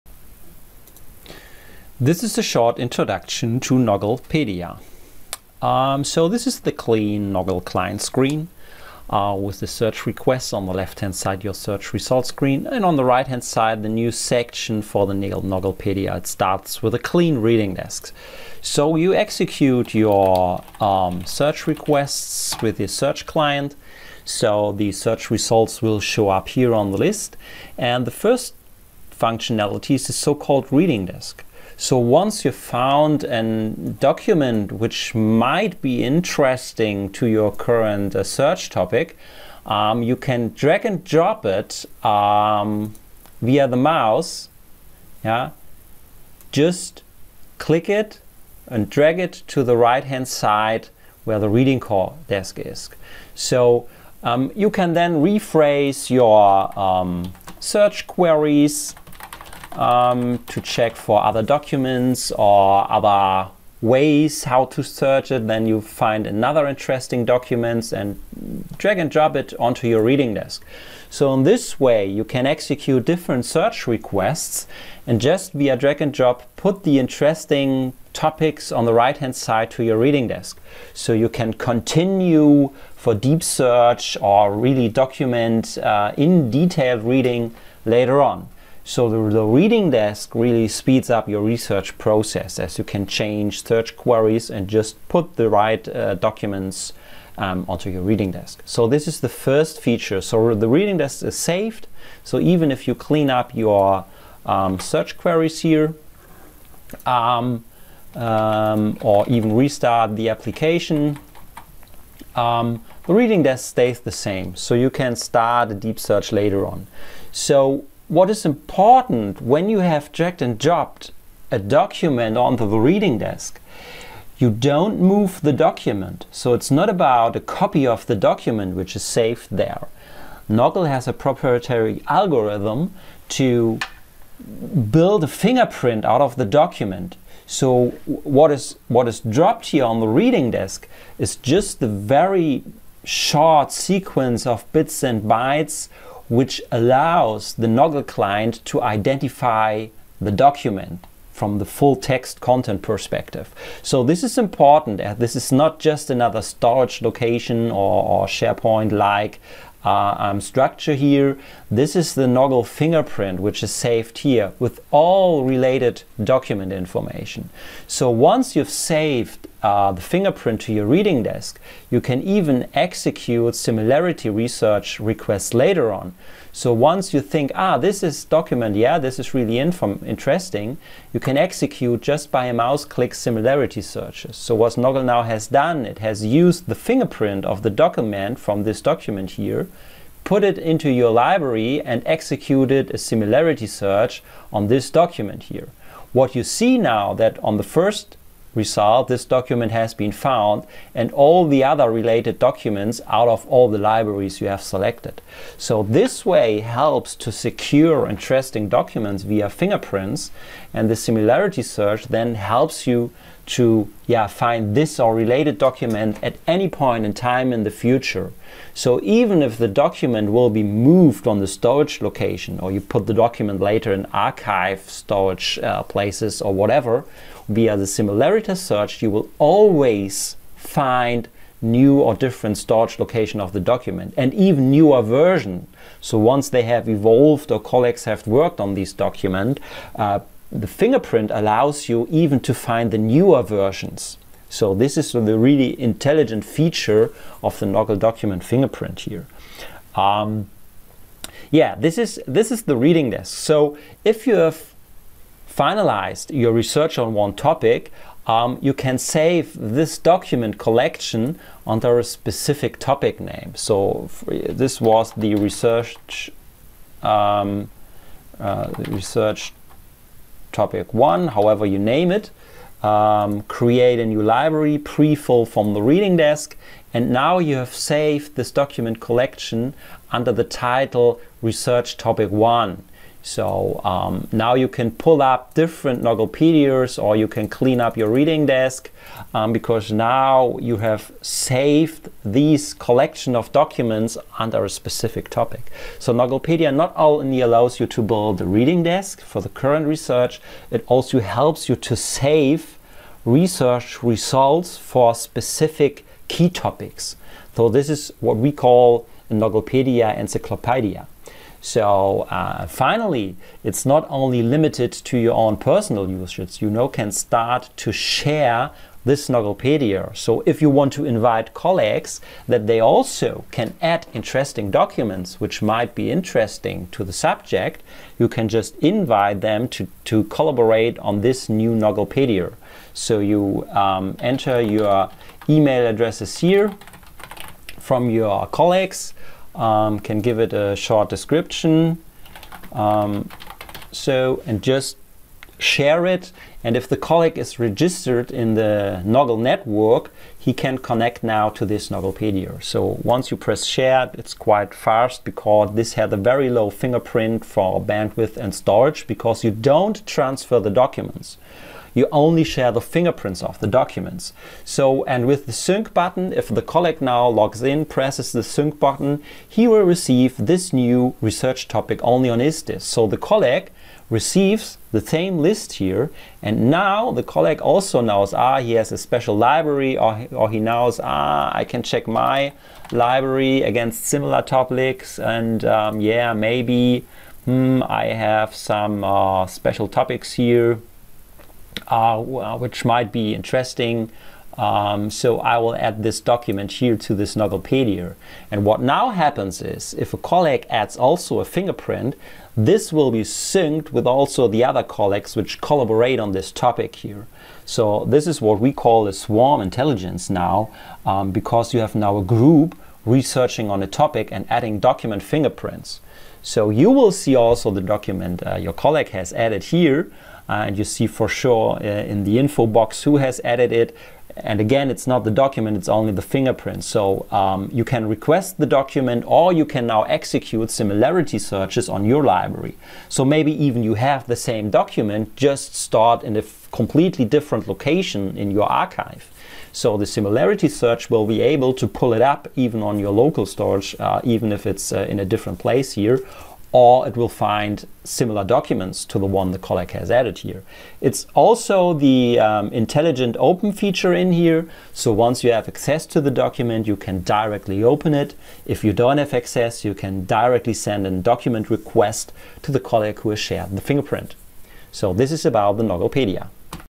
A Document Encyclopedia for the Digital Age
Nogglepedia is a collaborative document encyclopedia for the digital age. A Nogglepedia holds digital fingerprints of important documents from a particular branch of knowledge. Unlike a document library, which focuses on storage and document retrieval, Nogglepedia topics focus on factual document information about the subject for which the Nogglepedia is named.